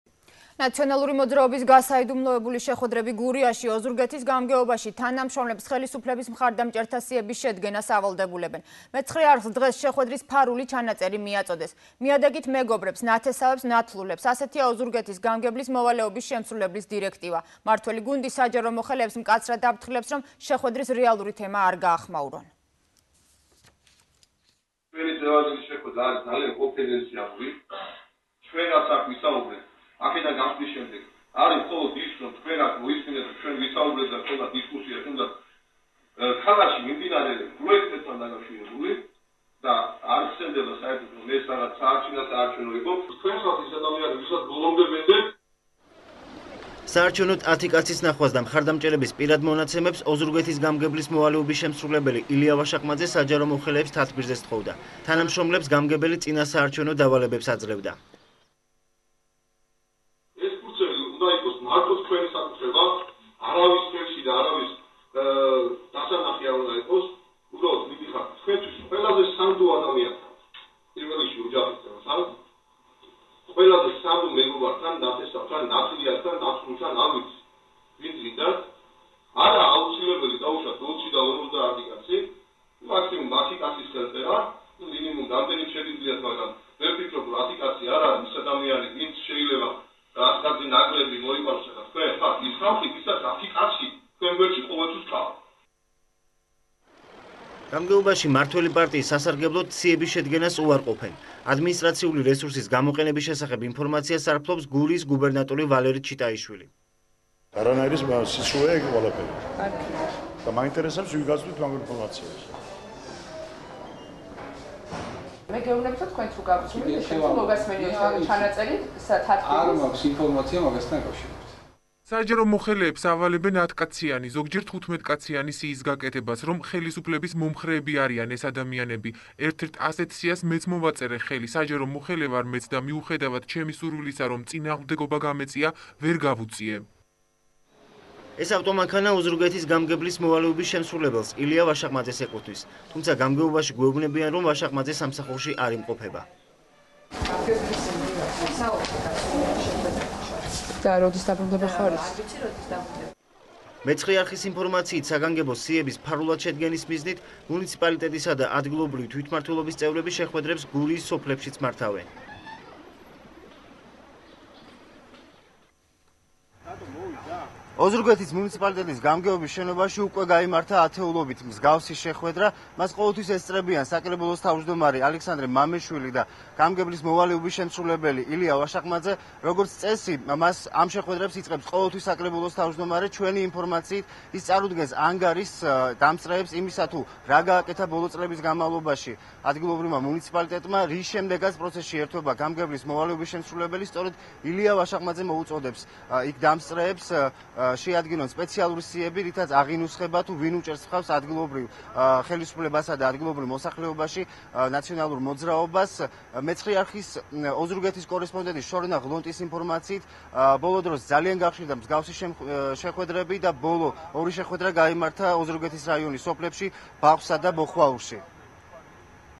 ახალი 9 | ფარული შეხვედრები | 24.05.12
„ნაციონალური მოძრაობის" გასაიდუმლოებული შეხვედრები გურიაში: ოზურგეთის გამგეობაში თანამშრომლებს ხელისუფლების მხარდამჭერთა სიების შედგენას ავალდებულებენ.  "მეცხრე არხს" დღეს შეხვედრის ფარული ჩანაწერი მიაწოდეს. "მიადექით მეგობრებს, ნათესავებს, ნათლულებს" - ასეთია ოზურგეთის გამგებლის მოვალეობის შემსრულებლის დირექტივა. მმართველი გუნდი საჯარო მოხელეებს მკაცრად აფრთხილებს, რომ შეხვედრის რეალური თემა არ გახმაურონ.